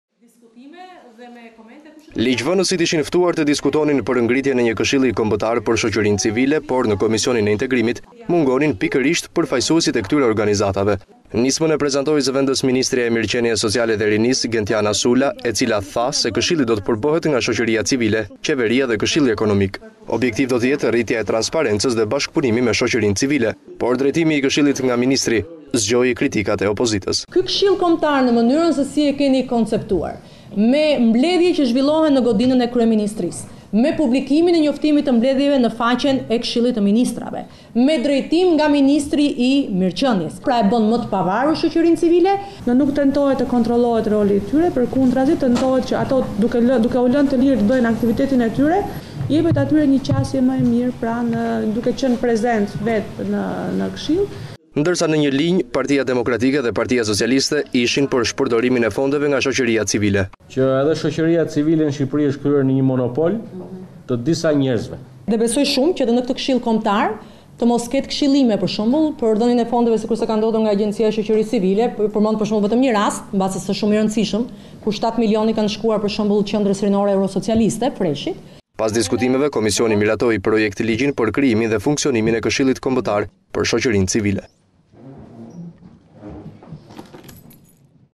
Komisioni i Integrimit, Këshilli i Shoqërisë Civile, debate për pavarësinë e tij- Ora News
Ligjvënësit ishin ftuar për të diskutuar për ngritjen e një Këshilli Kombëtar për Shoqërinë Civile, por në Komisionin e Integrimit mungonin pikërisht përfaqësuesit e këtyre organizatave.
Nismën e prezantoi zv/ministrja e Mirëqenies Sociale dhe Rinisë Gentjana Sula, e cila tha se këshilli do të përbëhet nga Shoqëria Civile qeveria dhe Këshilli Ekonomik. Objektiv do të jetë rritja e transparencës dhe bashkëpunimi me Shoqërinë Civile, por drejtimi i Këshillit nga ministri zgjoi kritikat e opozitës.